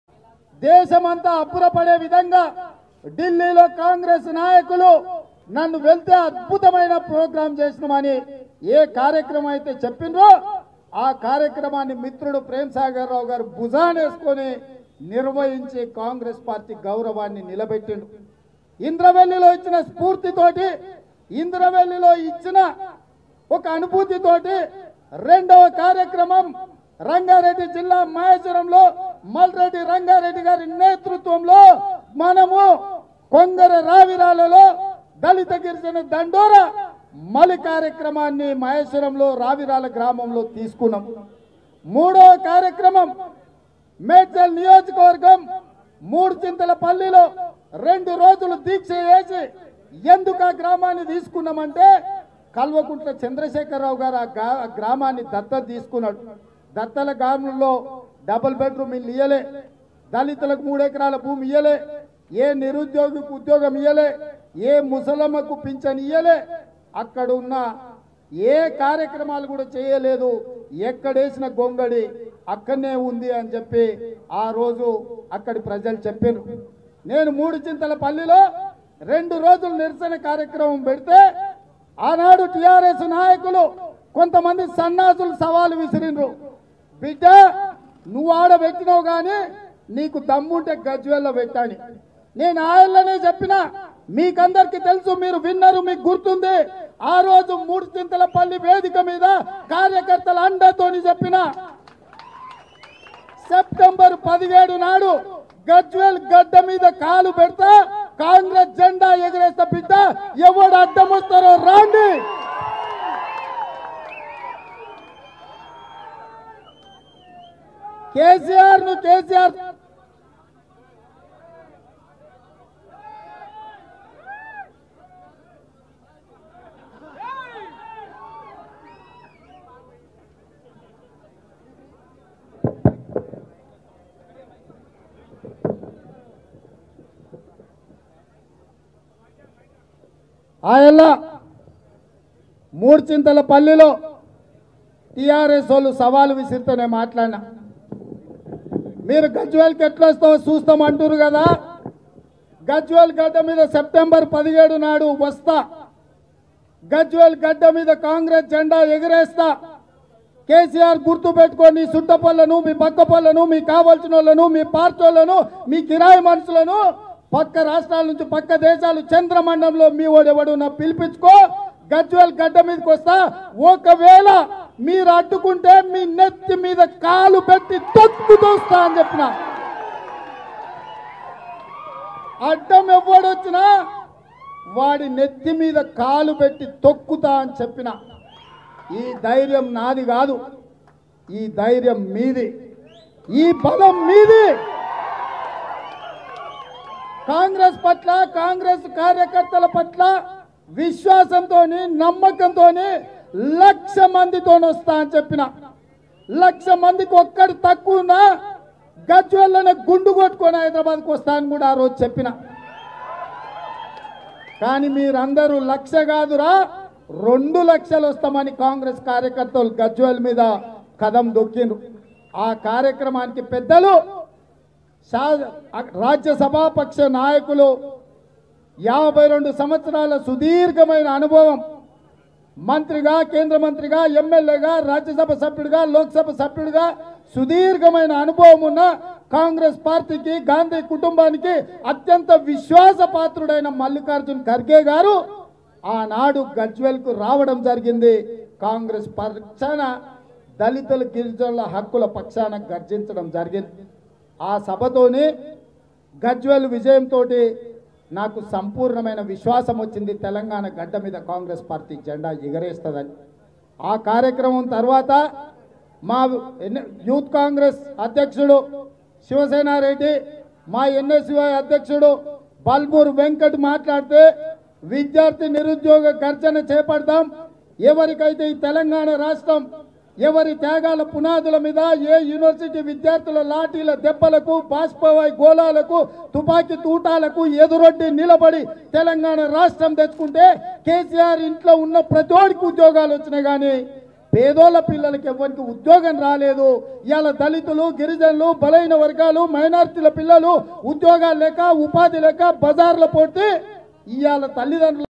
గద్వేల్ గడ్డపై కాంగ్రెస్ జెండా ఎగురవేస్తాం | TPCC Chief Revanth Reddy LIVE | Congress Joinings
LIVE🔴-  PCC Chief Revanth Reddy LIVE | Congress Joinings | Prime9 News